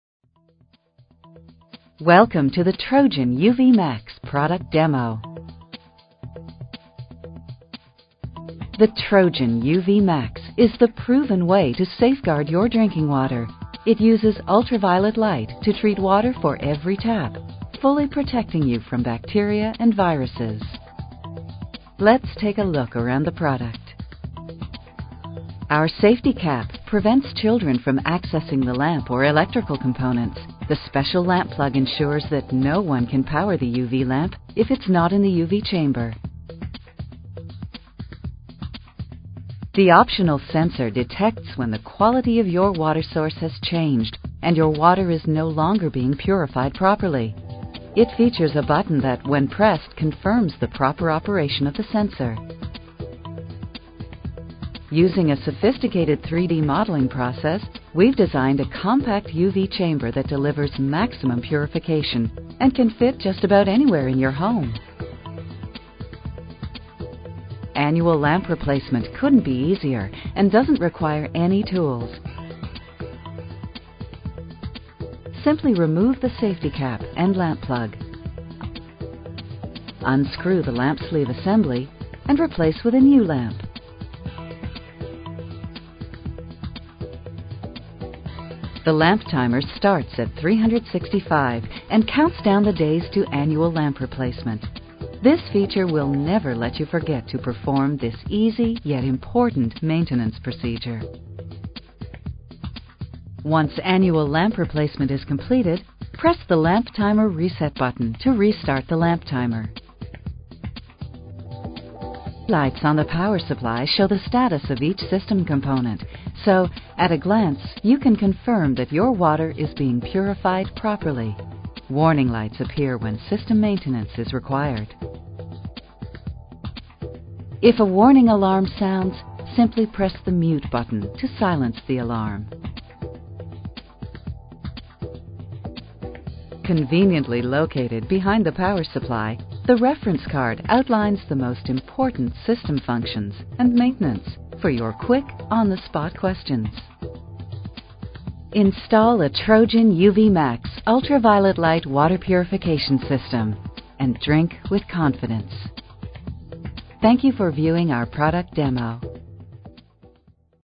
Trojan UVMAX UltraViolet Systems
Trojan UVMAX UltraViolet Systems produces ultraviolet light that eliminates waterborne pathogens by penetrating the cell wall of bacteria and viruses. This deactivates the DNA, and destroys the cells ability to reproduce. This effectively removes harmful bacterias and viruses from your water.